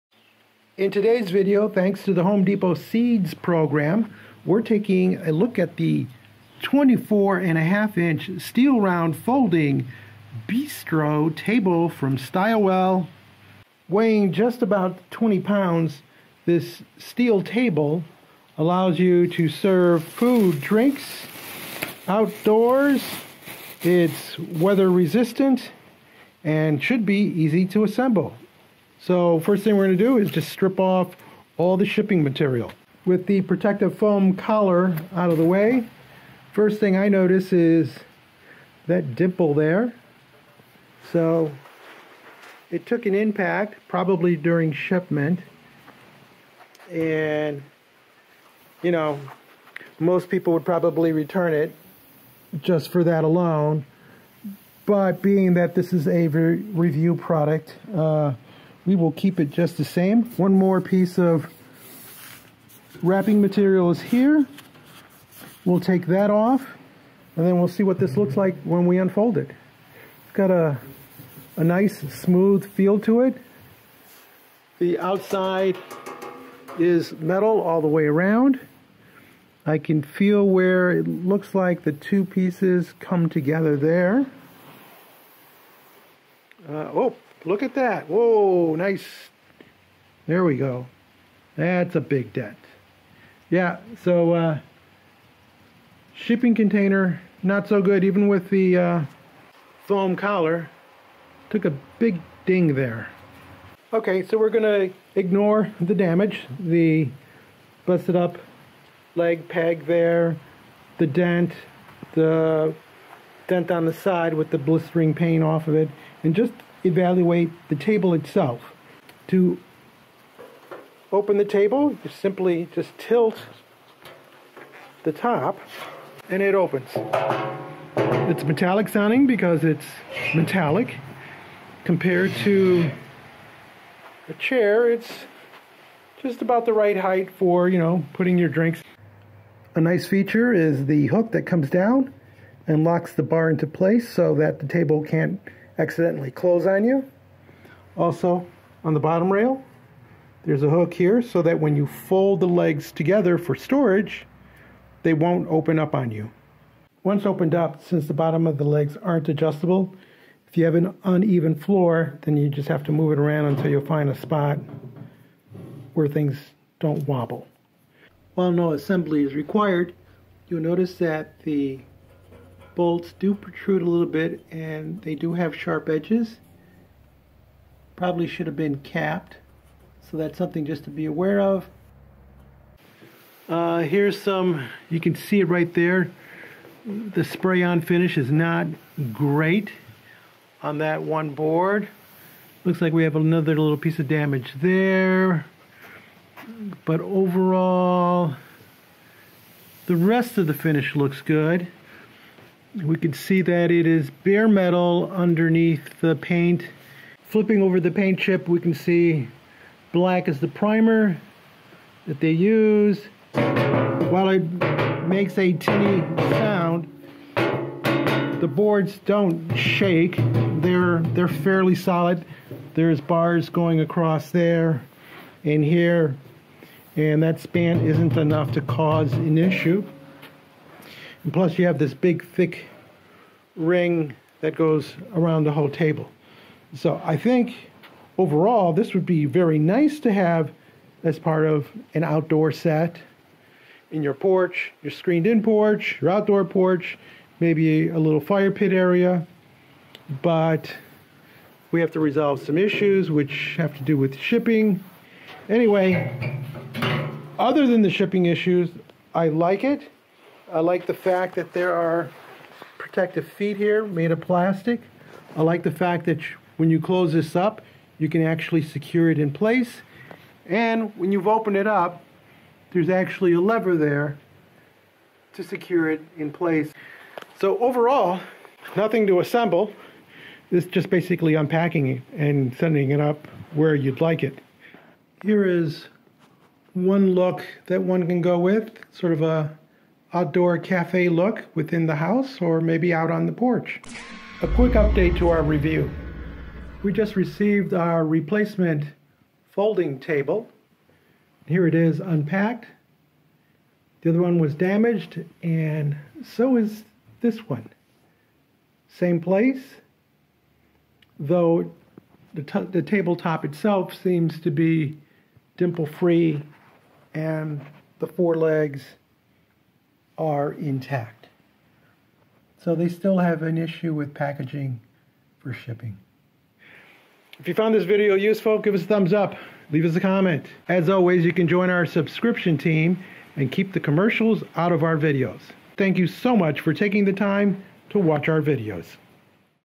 StyleWell Steel Bistro Table
StyleWell offers a steel bistro table that is at home indoors or outside. It comes pre assembled so all you do is unfold and enjoy. While this out of the box furniture is durable there may be issues you need to be aware of. Follow along as we unbox two bistro tables.

Product provided by Home Depot Seeds program in return for an unbiased written review with no other obligation or compensation. Home Depot has nothing to do with the making of this video.